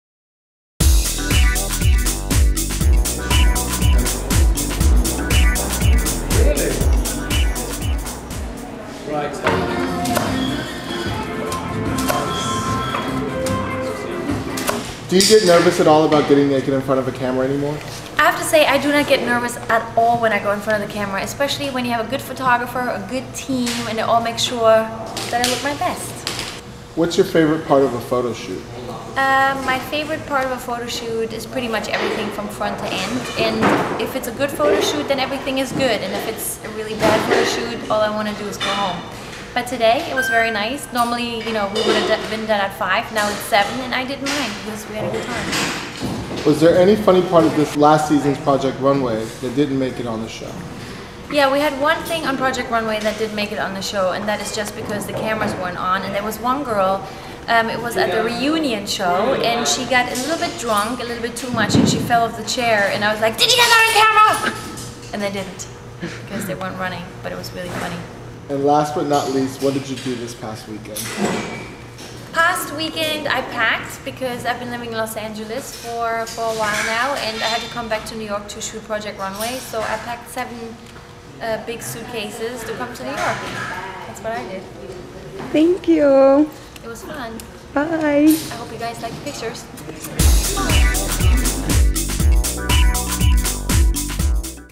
CELEB: Heidi Klum Behind the Scenes at the Naked Shoot
Heidi Klum talks about getting naked, photo shoots, and Project Runway while behind the scenes at the Naked shoot.